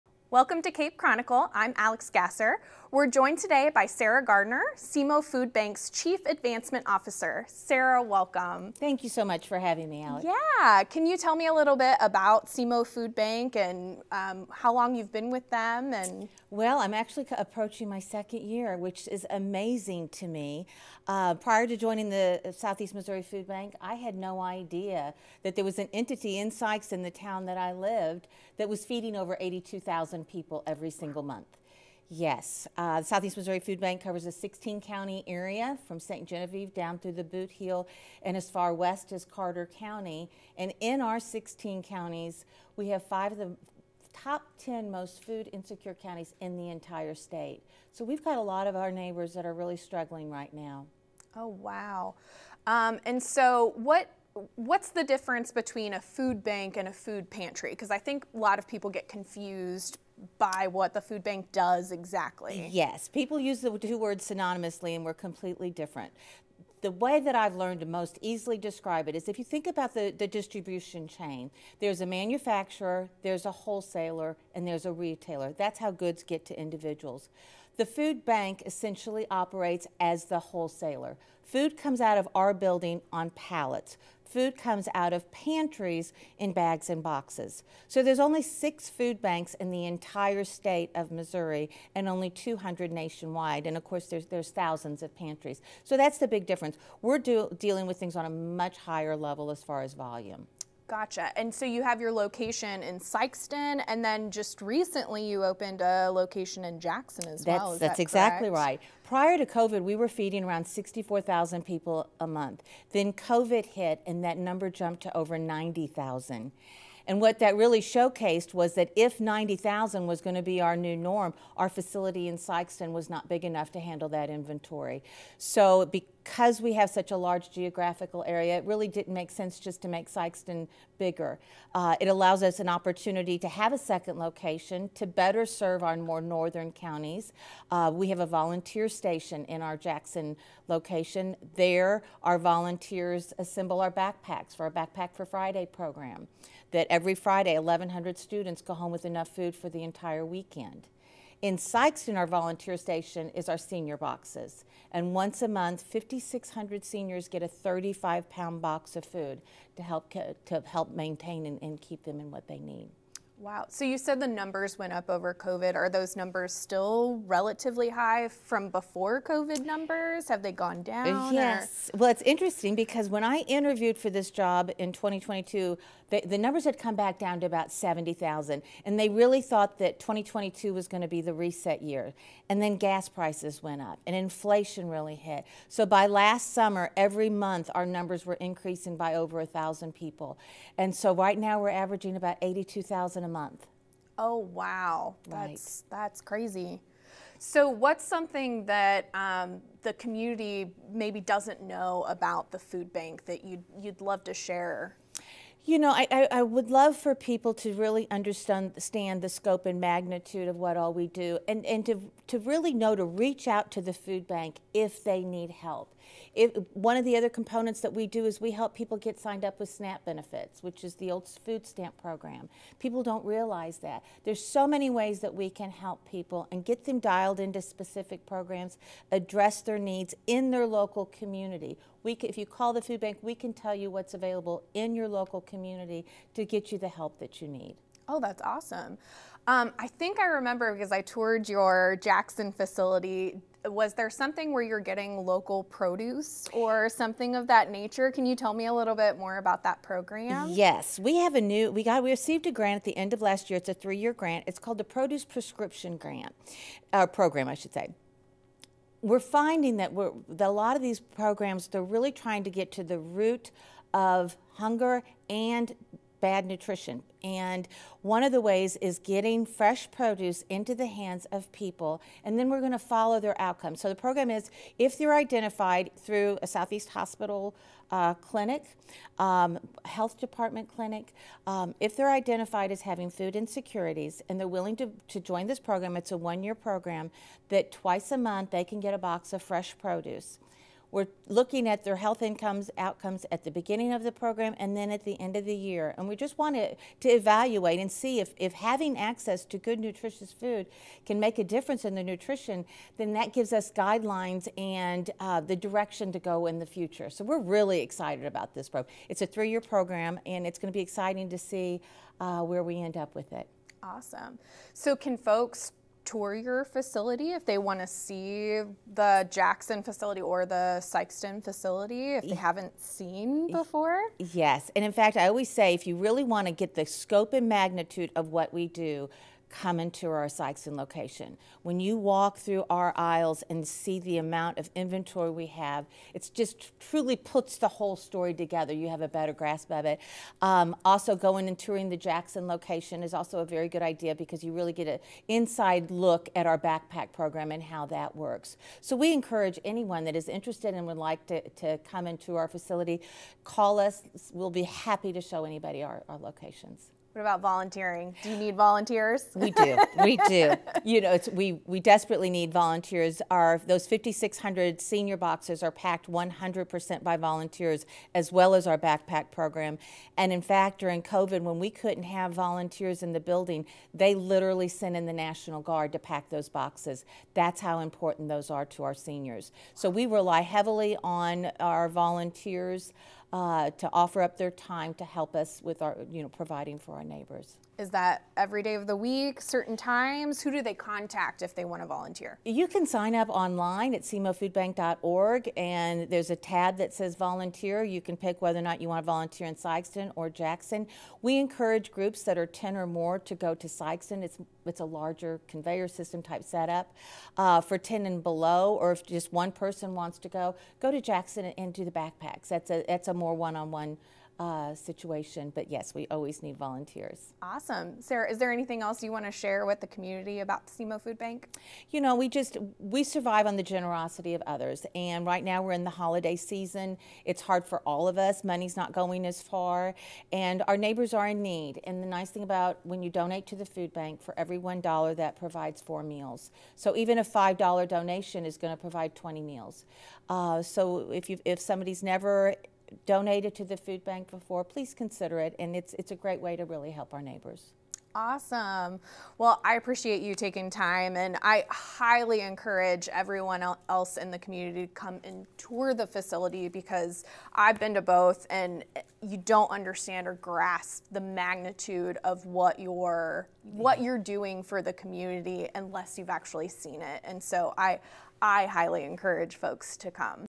Talking SEMO Food Bank with Sarah Garner | Cape Girardeau
NOV. 30, 2023 - Doing a little throwback to when host Alix gasser sat down with Sarah Garner, Chief Advancement Officer of the SEMO Food Bank! In this segment, Garner shares her experience joining the team 2 years ago, and how the impact the SEMO Food Bank has on the region has impacted her life and outlook.